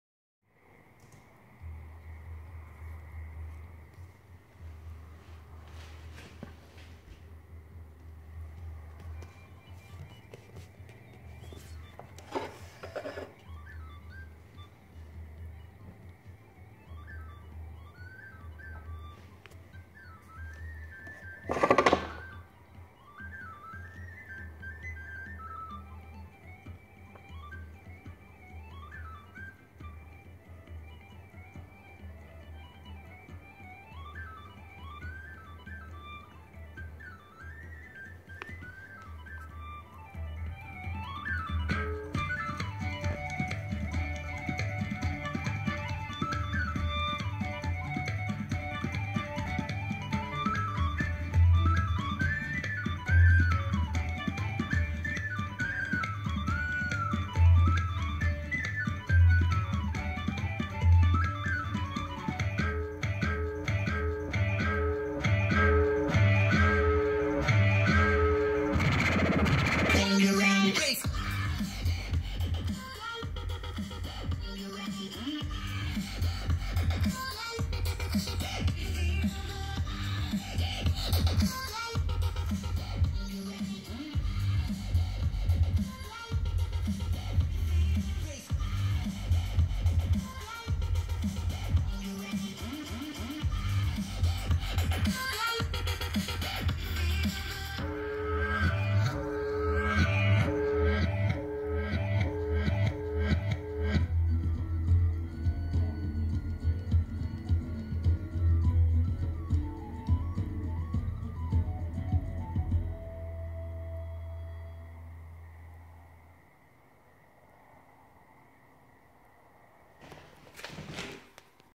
St Patrick's Day beginner Light o Rama show
Light and music show using Light O Rama. Set up during a wind and rain storm so it definitely didn't get the result I was intending. But it was still fun and the neighborhood actually enjoyed it.